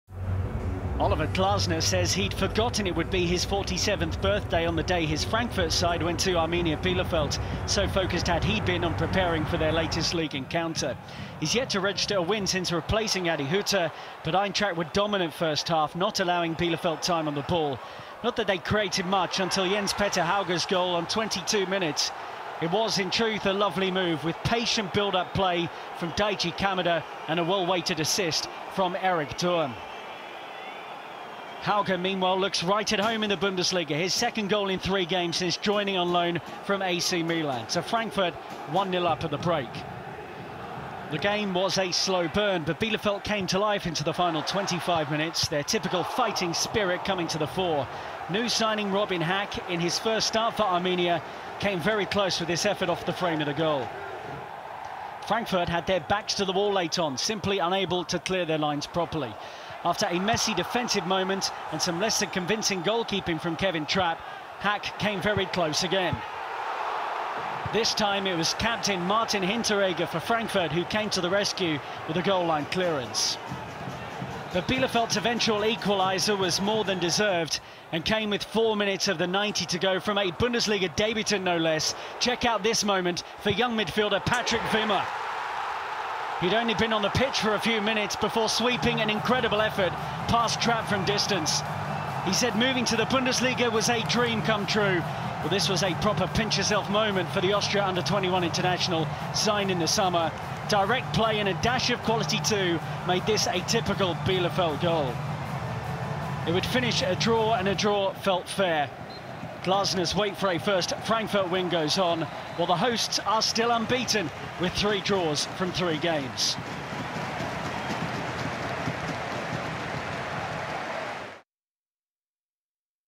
Arminia Bielefeld - Eintracht Frankfurt 1-1 | Highlights | Matchday 3 – Bundesliga 2021/22
DSCSGE | Highlights from Matchday 3!

Watch the Bundesliga highlights of Arminia Bielefeld vs. Eintracht Frankfurt from Matchday 3 of 2021/22 season!

Goals: 0-1 HAUGE (22’), 1-1 WIMMER (86’)